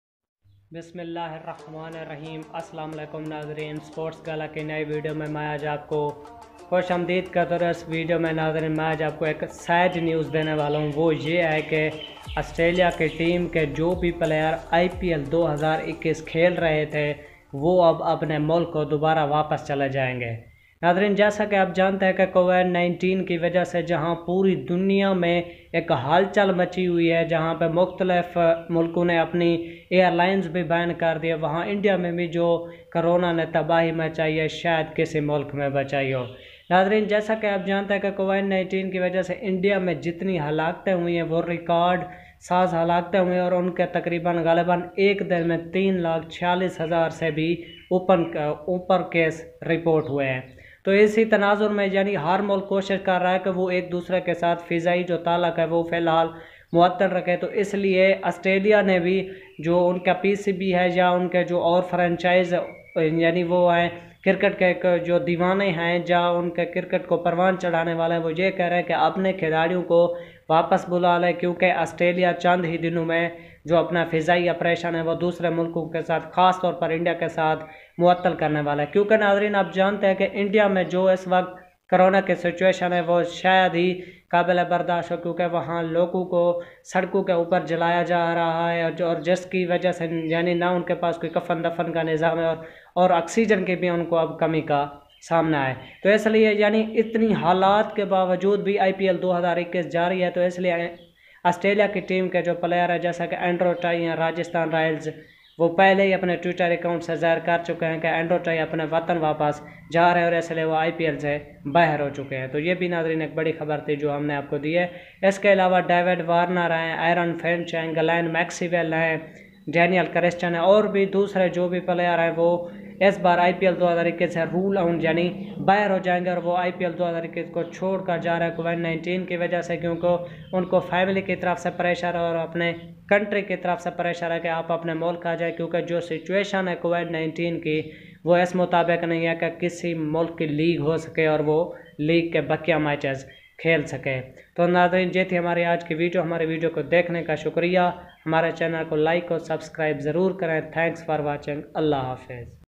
All Australian Player's Ruled out From IPL 2021//IPL 2021 Has Been Postponed.
Foreign Player's Ruled out From IPL 2021//All Australian Player's are Out From IPL.